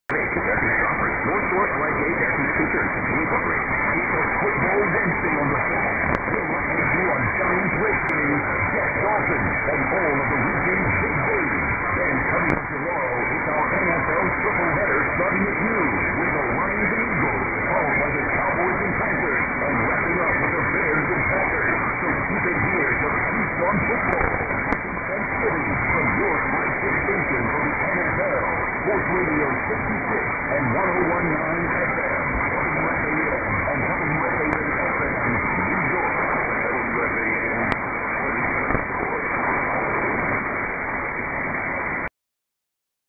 MW DX: WFAN New York 660 kHz received in Germany
WFAN New York 660 kHz received in Germany on 26 November 2015 at 0400 UTC.

Receiver: Elad FDM-S2 with Wellbrook ALA1530 active loop antenna (indoors)

Distance: 6155 km  -  Power: 50 kW